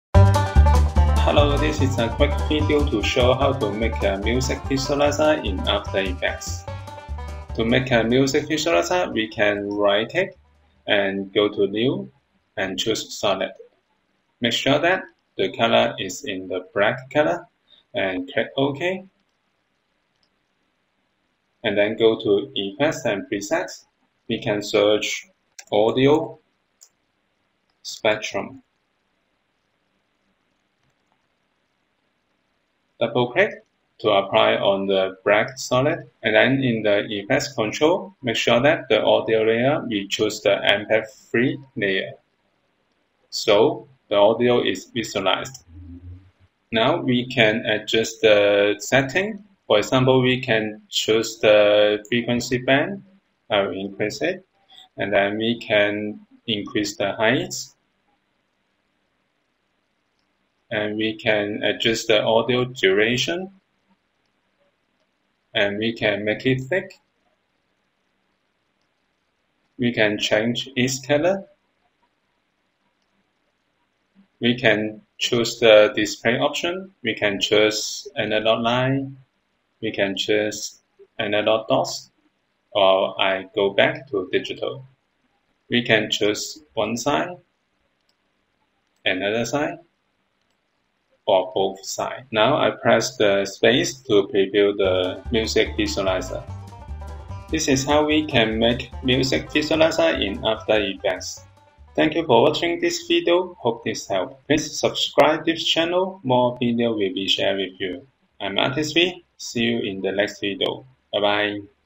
After Effects Visualizer Tutorial for Audio, Music, Sound, Waveform , Spectrum
After Effects Visualizer Tutorial for Audio, Music, Sound, Waveform , Spectrum. This is free audio visualiser made by After Effects 2023, we can import an audio file  like as mp3 into AE, then apply Audio Spectrum Effects to make the Visualizer. Steps are easy to follow by After Effects Beginners. Hope this hopes.

Timestamp
0:00 Intro – After Effects Audio Visualizer
0:10 Add Solid Layer
0:28 Apply Audio Spectrum
0:40 Audio Spectrum Effects Setting
1:17 Audio Spectrum Display Options